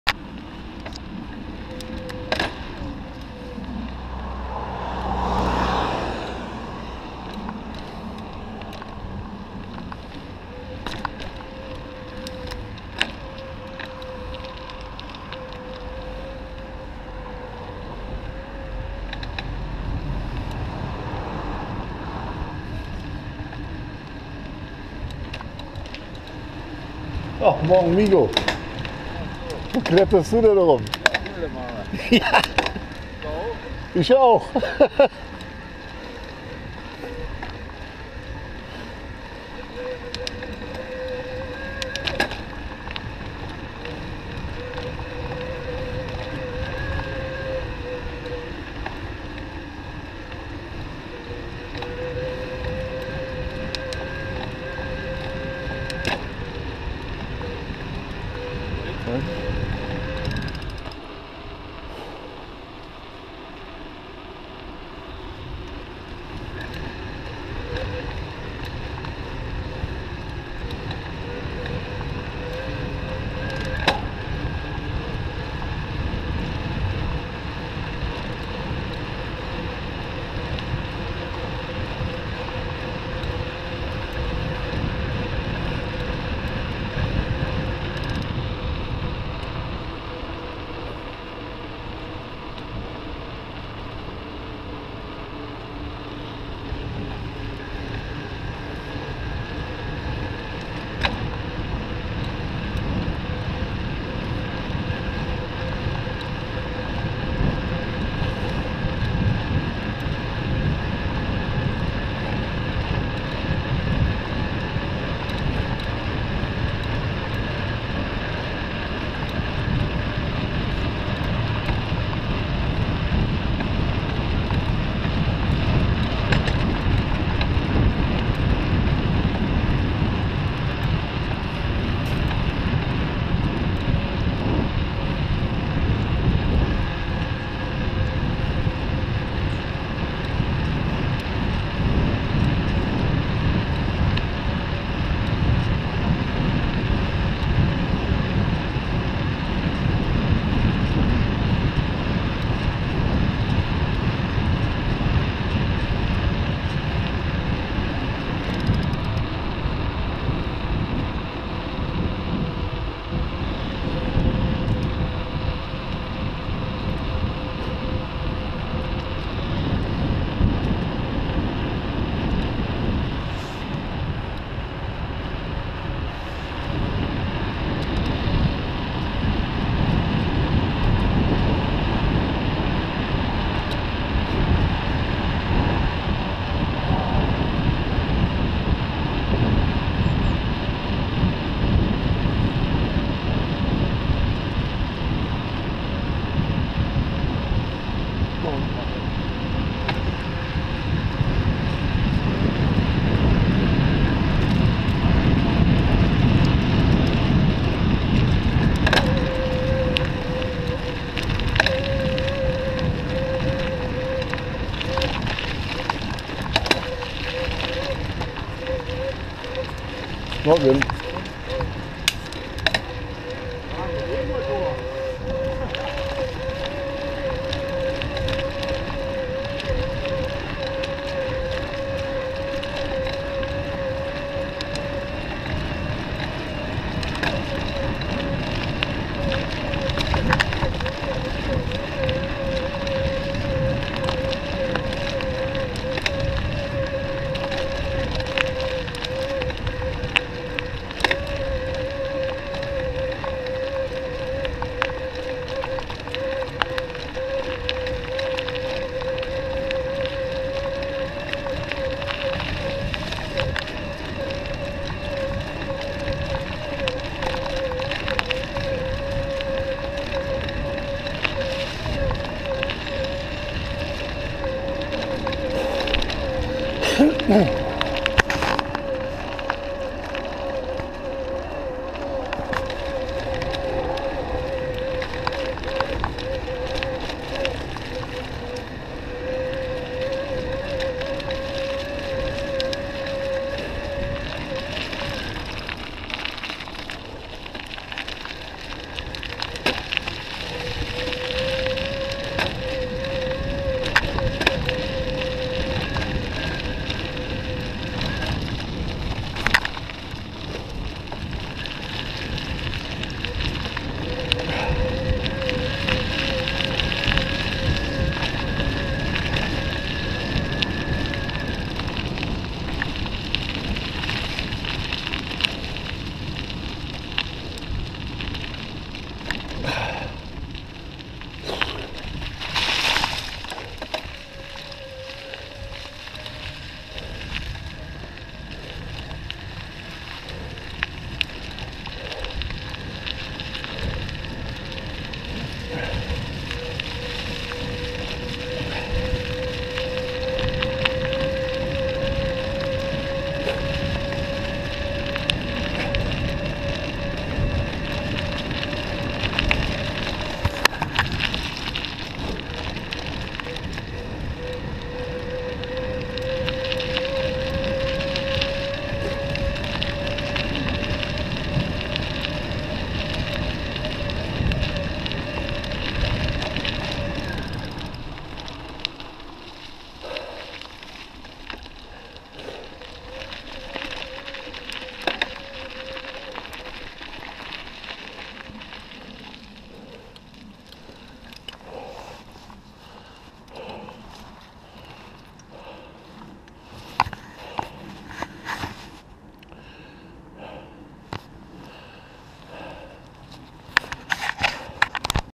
HAIBIKE-E Bike Bosch-1/12-ANDERNACH-NAMEDY-KULTUR-RAD-TOUREN.
Vom Rhein zur Aussichtskanzel.